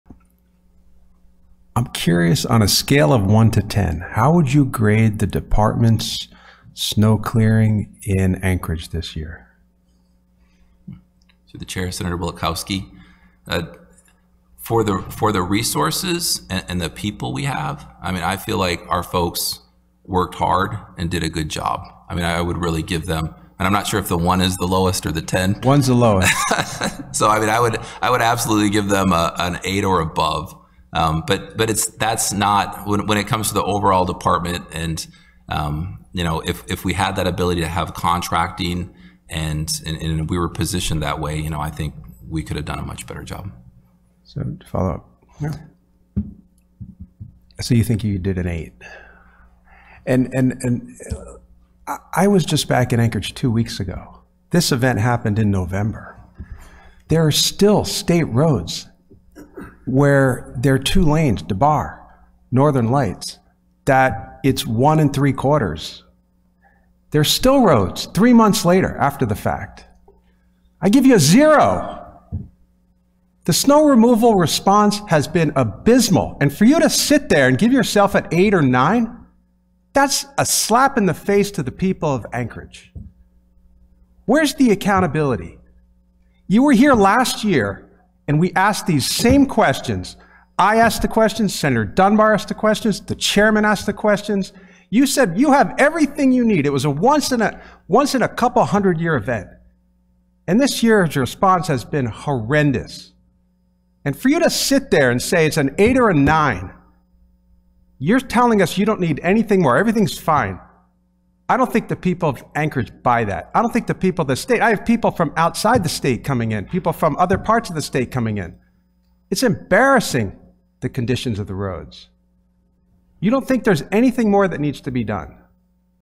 Sen. Wielechowski responding to AK DOT&PF
Sen. Bill Wielechowski responding to Ryan Anderson, the Commissioner of Alaska Department of Transportation and Public Facilites, comments about the quality of job the department has done with snow plowing this winter in Anchorage.